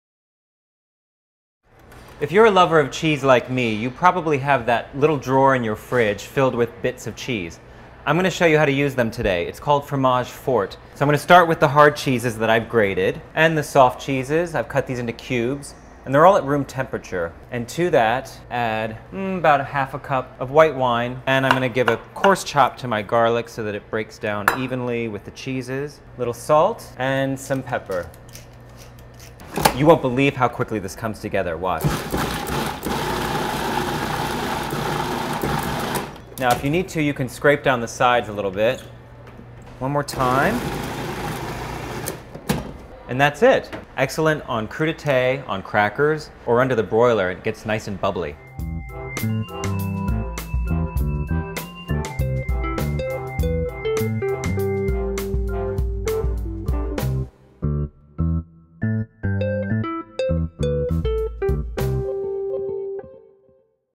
How to Make Fromage Fort - Martha Stewart
Greg Lofts makes fromage fort, a rich spread made from leftover cheeses.